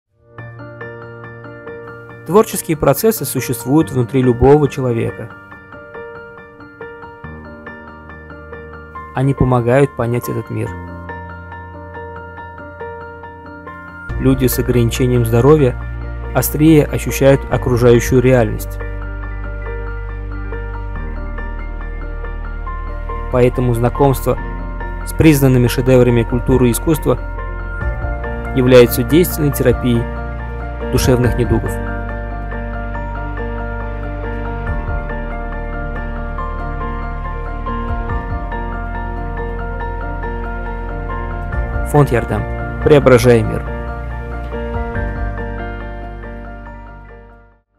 Поход в "Хазинэ"
Реабилитанты проекта «Видеть и слышать сердцем» посетили Национальную художественную галерею "Хазинэ", где не только познакомились с творениями художников, но и сами учились сделать что-то неповторимое. Мастер-класс по художественной лепке прошёл на ура...

В реабилитационном центре фонда «Ярдам-Помощь» курсы по проекту «Видеть и слышать сердцем» проходят при поддержке БФ «ТАТНЕФТЬ».